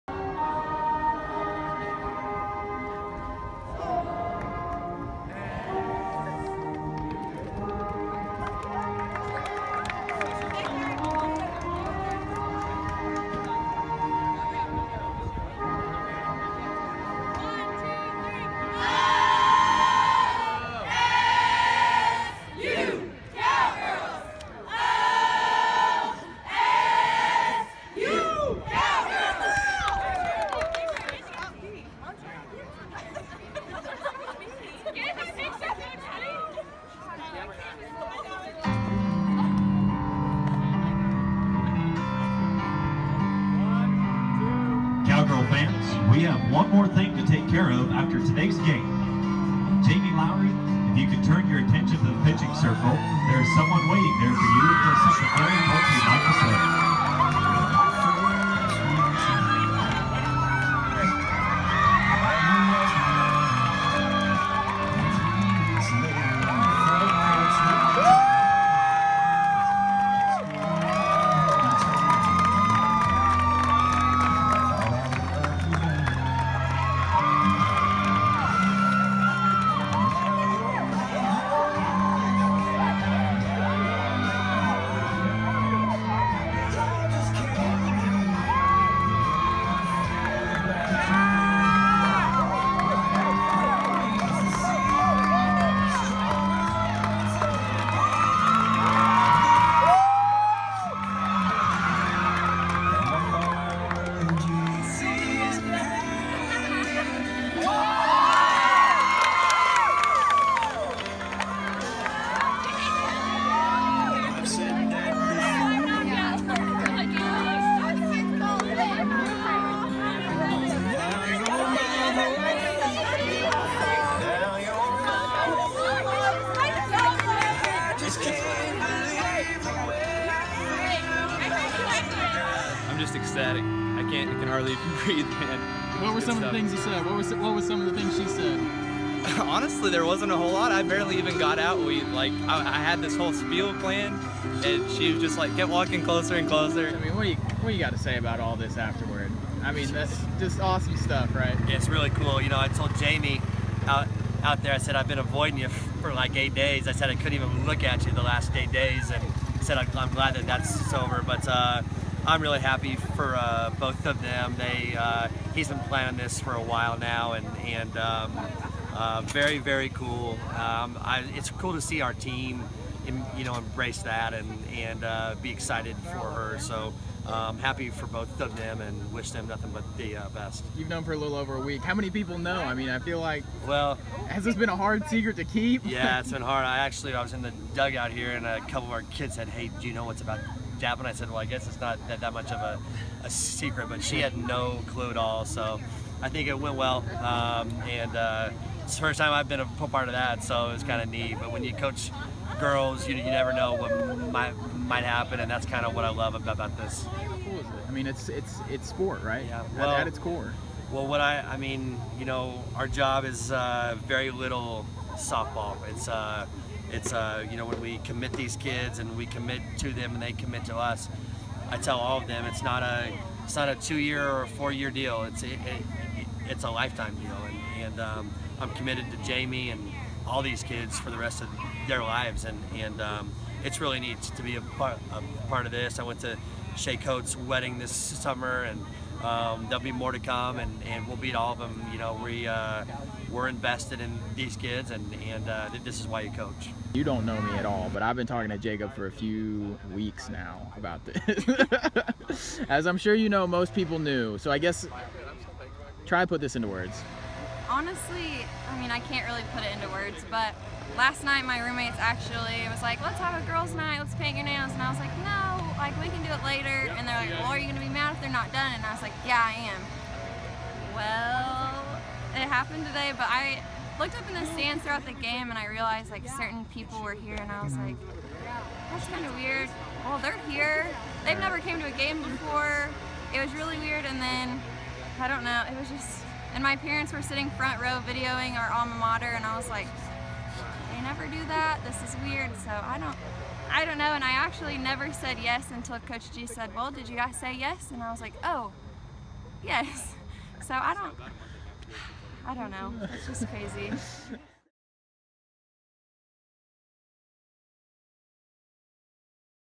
Cowgirl Softball Postgame Marriage Proposal
After Oklahoma State's 11-5 exhibition win against Butler Community College on Thursday, Jacob Overton proposed to Jamie Lowrie, his girlfriend and Cowgirl senior outfielder. Here is how it all happened and what they and coach Kenny Gajewski said afterward.